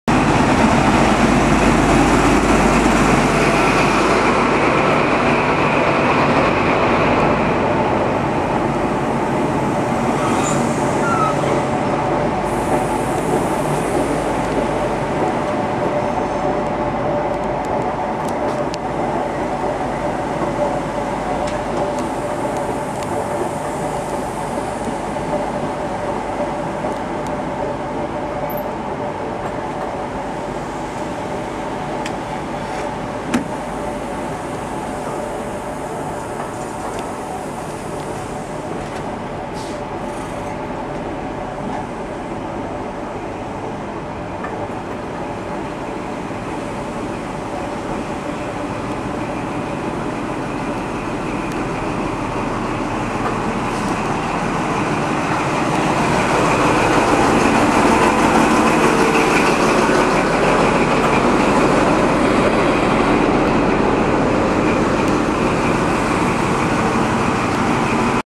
East Midlands Trains 125 HST arriving into London St Pancras.
21/02/09 - East Midlands Trains 125 HST arriving into London St Pancras railway station.